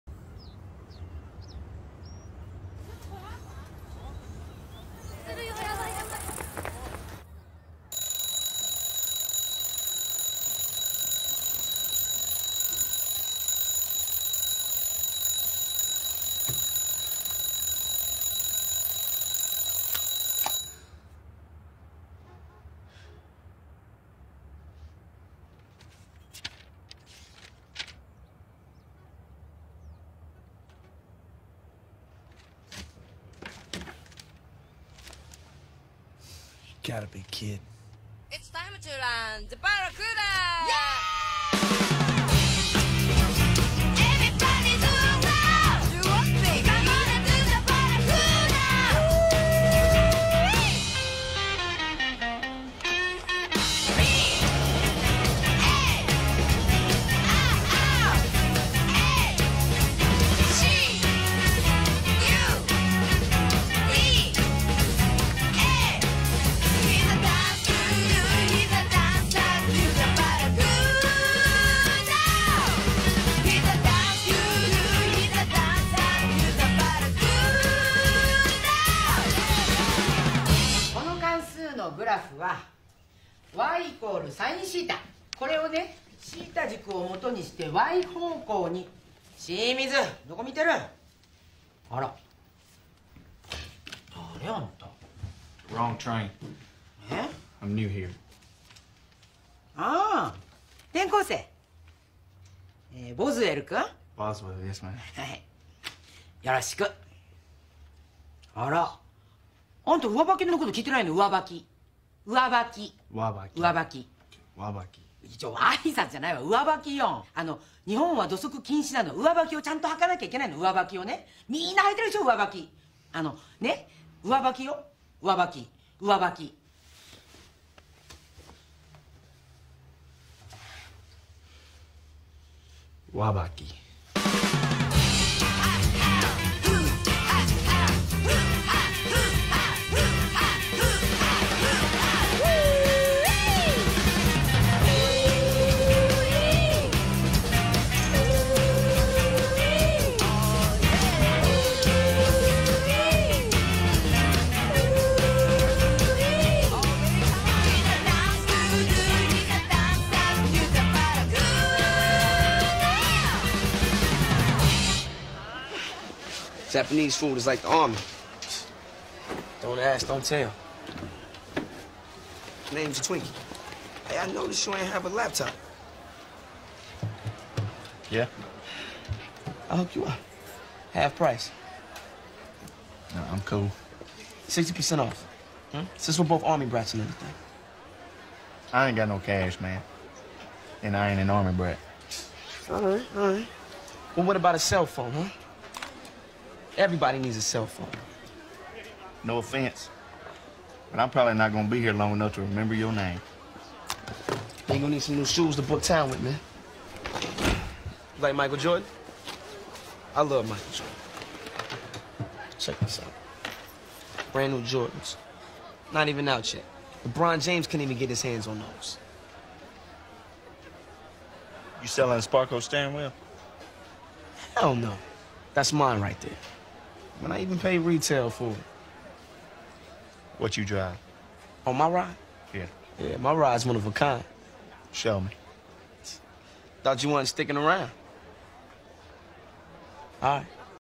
First day of school in Tokyo....FUCKING WABAKI!
hi dis wat happens when u first come to a jap school. (: